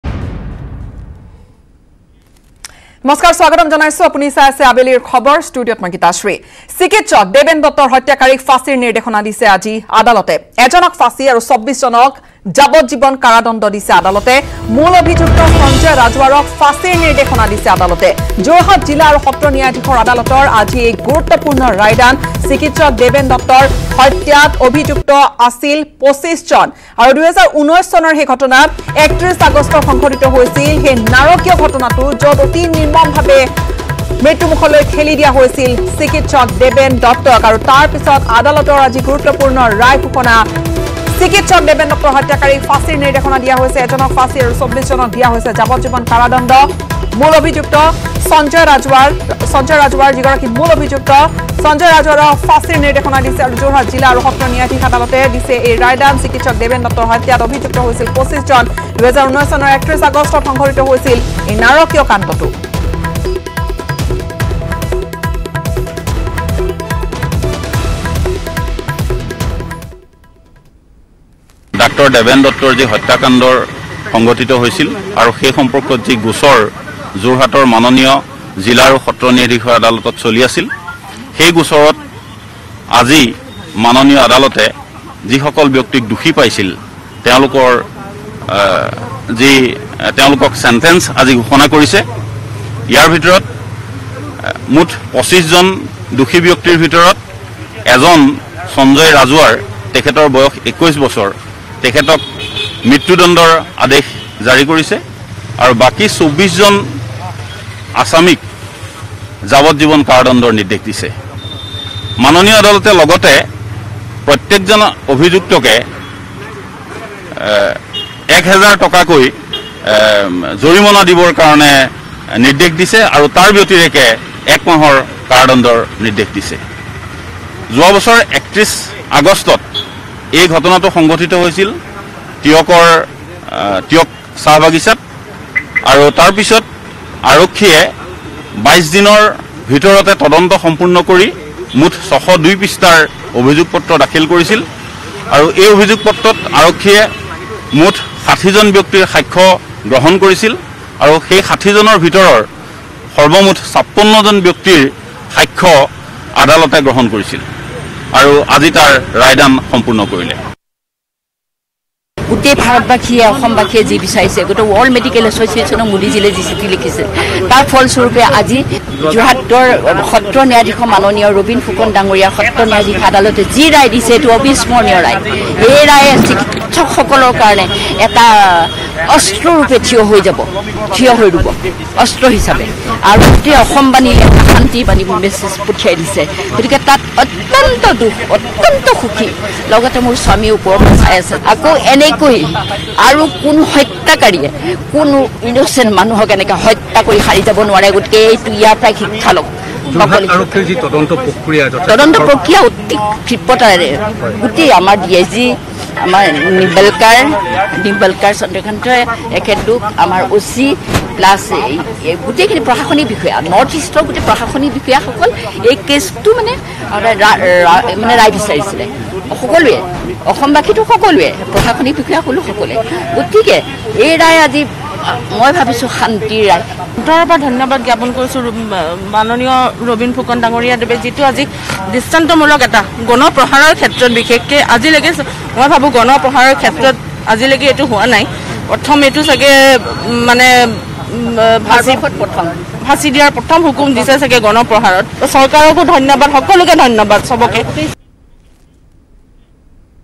Deben Duttaৰ হত্যাকাণ্ডৰ এজন অভিযুক্তক ফাঁচীৰ নিৰ্দেশ আদালতৰ
চিকিৎসক Dr Deben Duttaৰ হত্যাকাণ্ড গোচৰৰ দোষীৰ শাস্তি ঘোষণা আদালতৰ । এজনক ফাঁচি, আৰু আন ২৪জনক যাৱজ্জীৱন কাৰাদণ্ডৰ নিৰ্দেশ ।
২০১৯ চনৰ ৩১ আগষ্টত সংঘটিত এই   দেশ কঁপোৱা ঘটনাৰ । আৰু এই ঘটনাৰ ২২ দিনৰ পিছতে যোৰহাটৰ তদন্তকাৰী আৰক্ষীয়ে ৩২জন অভিযুক্তৰ বিৰুদ্ধে চাৰ্জশ্বিট দাখিল কৰিছিল। তাৰে ২৫জনক কৰা হৈছিল দোষী সাব্যস্ত ।